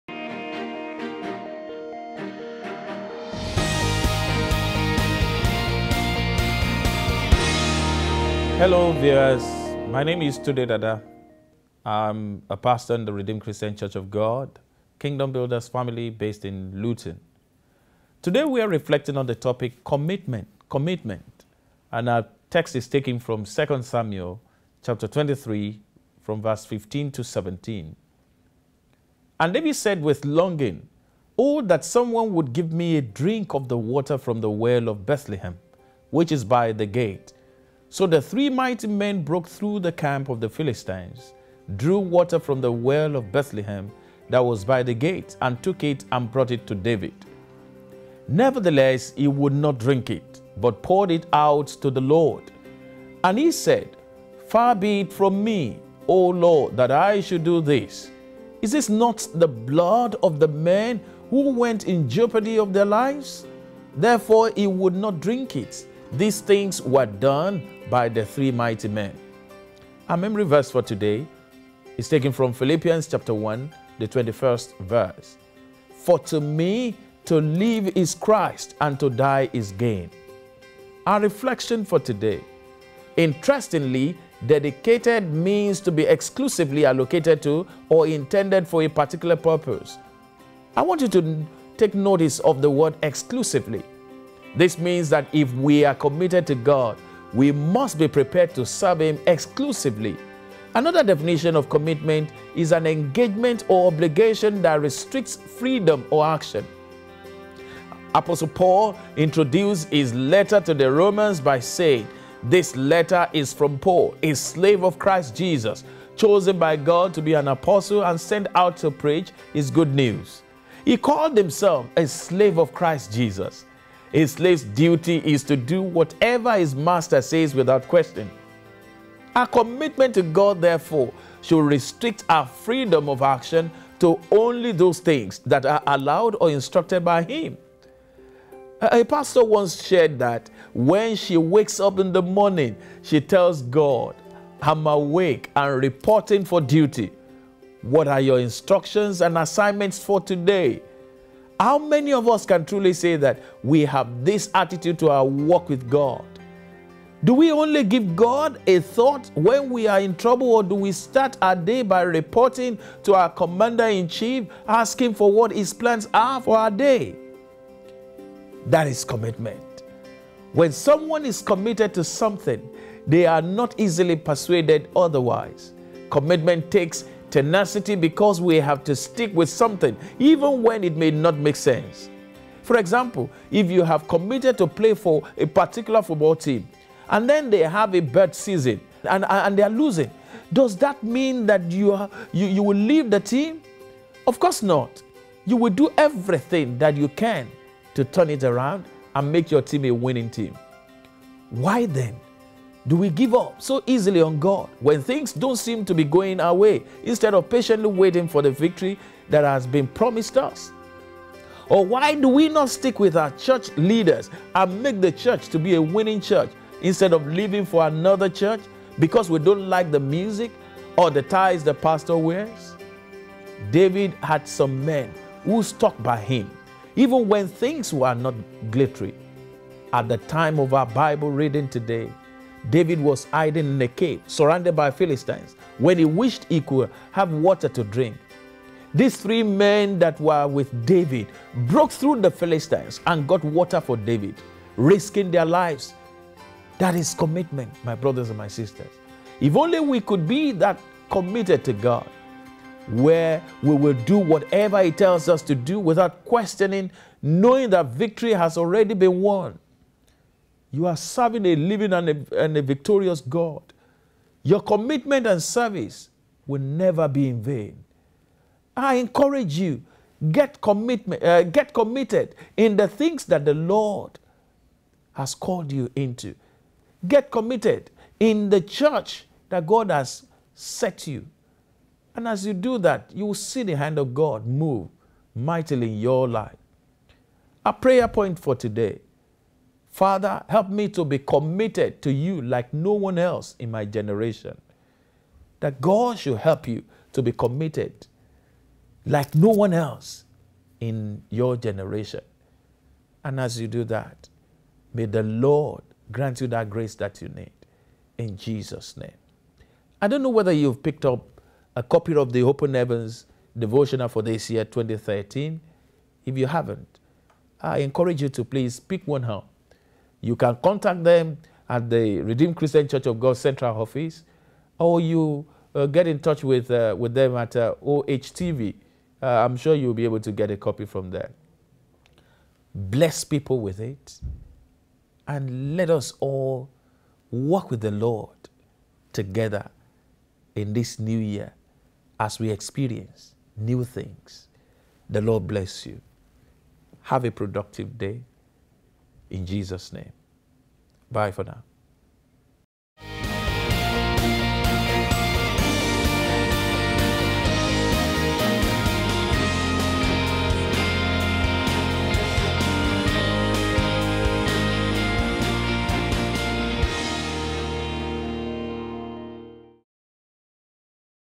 Open Heavens Reflections January 21st 2013 - Commitment
Reflections by Pastor Tunde Dada, Pastor of RCCG, Kingdom Builder's Family, Luton, UK from the Open Heavens Devotional written by Pastor E. A. Adeboye, General Overseer of the Redeemed Christian Church of God.